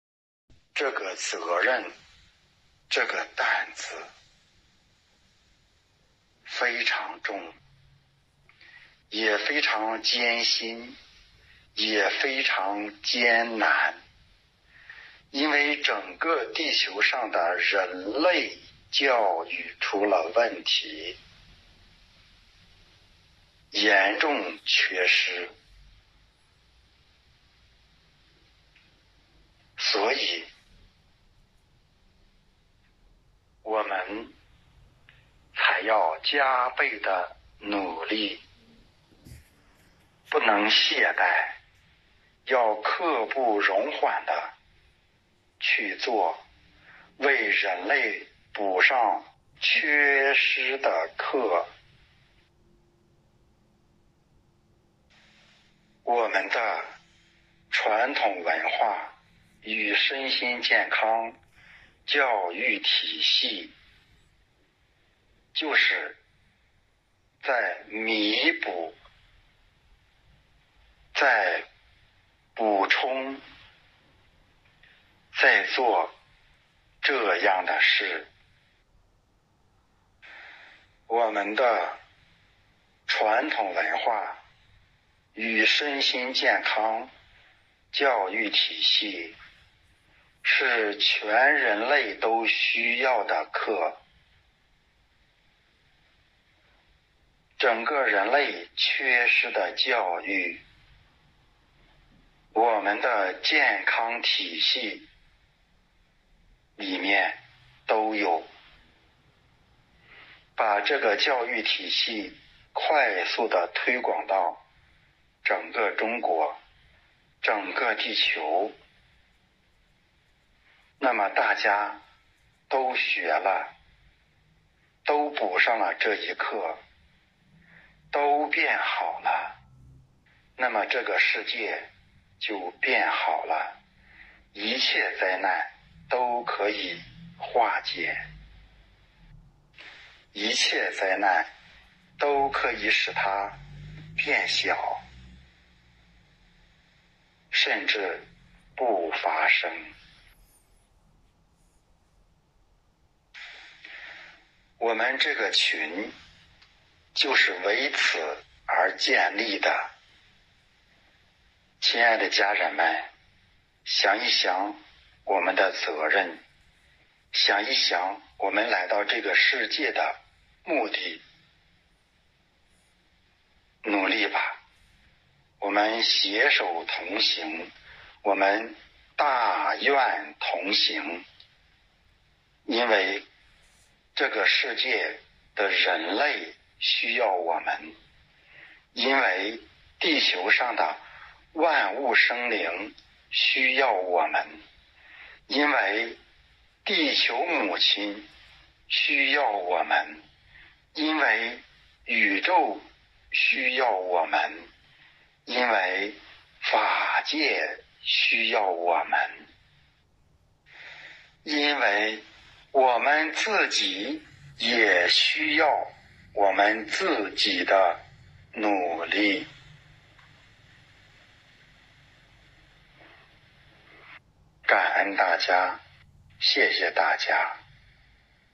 整个地球上的人类教育出了问题，严重缺失。我们是为了补上缺失的课。
摘自：赵宗瑞老师管理群讲话《为什么而来！》